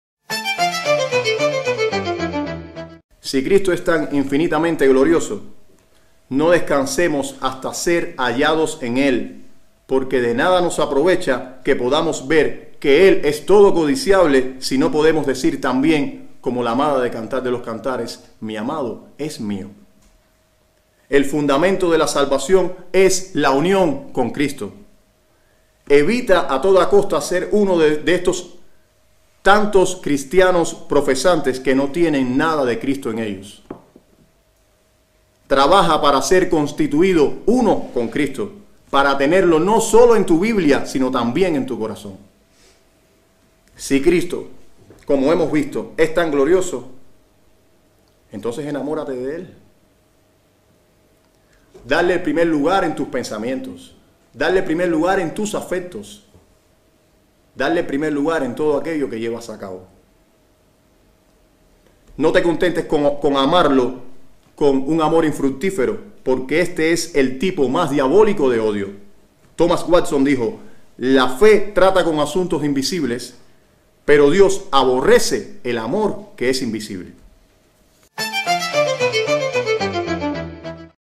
Muestra de "La gloria de Jesucristo"
Todos los verdaderos creyentes saben que Cristo es glorioso,​ pero muchos no conocen cuántos detalles bíblicos revelan Su gloria. ¿Los conoces tú?